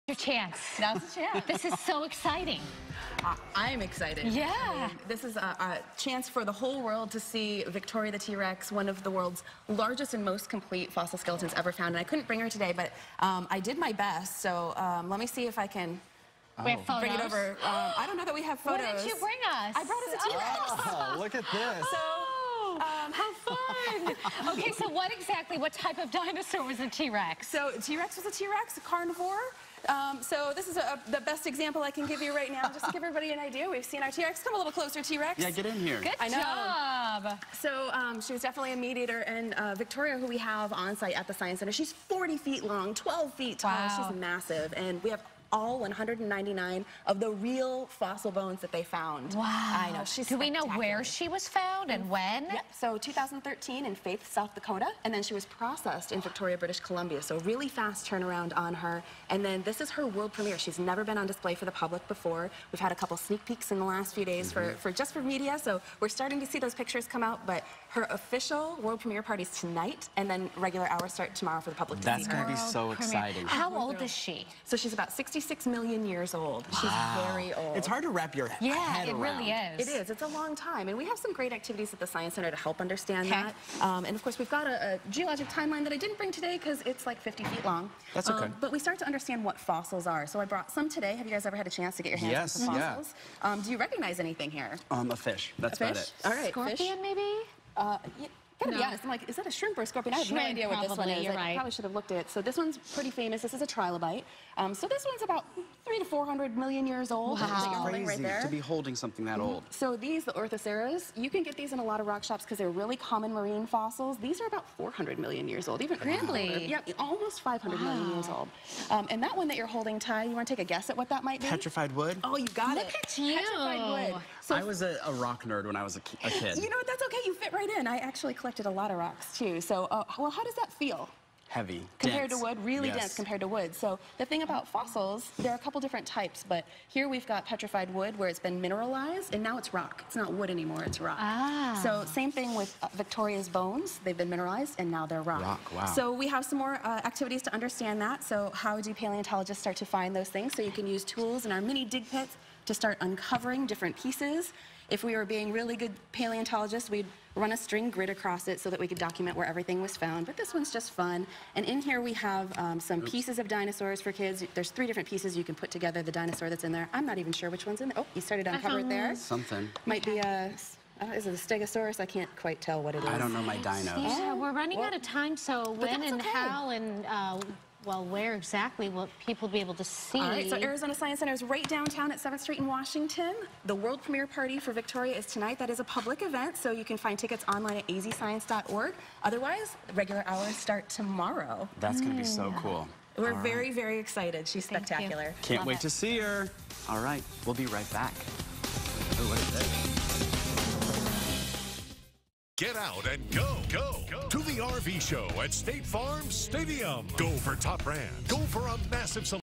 Fox10 Science Saturdays: Meet Victoria, a REAL T. rex dinosaur
Born 66 million years ago, meet Victoria the T. rex, a newly discovered dinosaur now on display in a world premiere exhibition. Experience her life in this state-of-the-art exhibition through 3D projection mapping, holograms, and interactive displays.  Only at Arizona Science Center for a limited time.